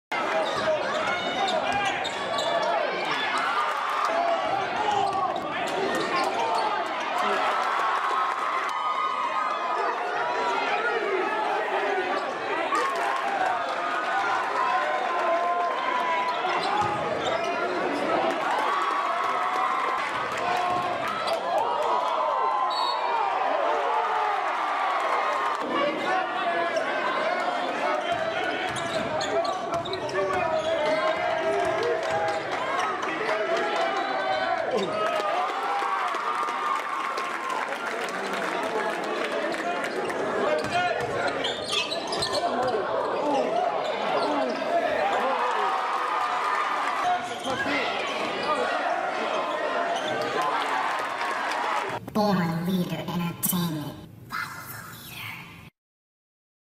Quade Green 15 pts 7 asst 6 rebs In Win Over Carroll
Quade Green 15 pts 7 asst 6 rebs In Win Over Carroll in 2017 Phila CL Quater Finals